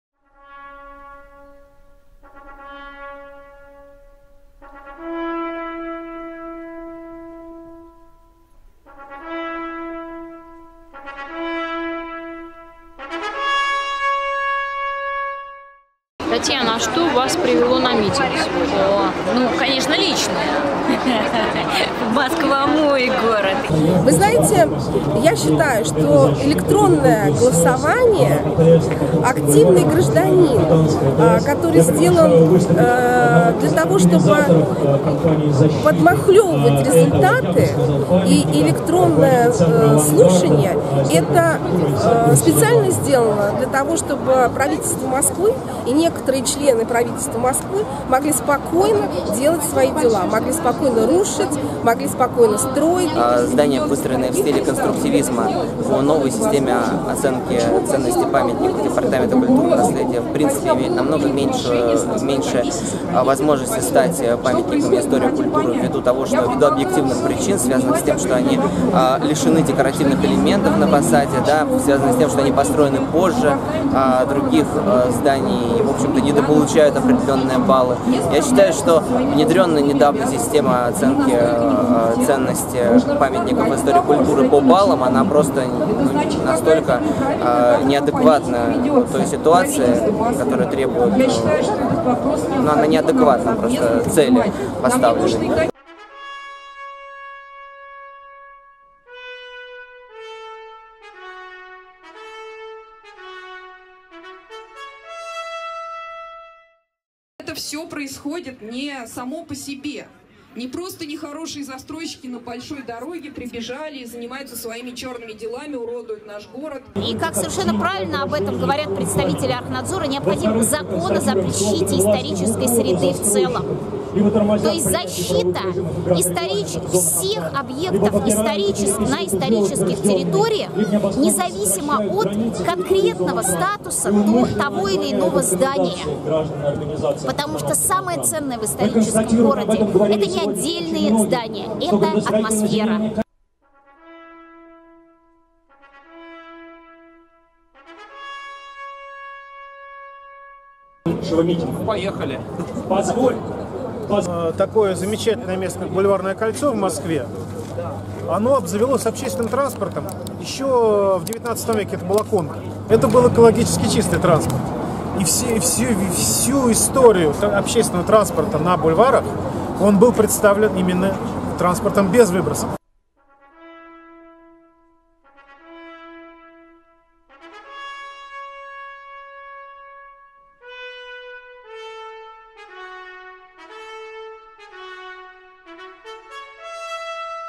Москвичи в защиту исторической Москвы. Часть 2
Описание26 мая 2016 г. на Суворовской площади в Москве прошел общегородской градозащитный митинг, главным лозунгом которого стал: "Москве -  статус исторического города!".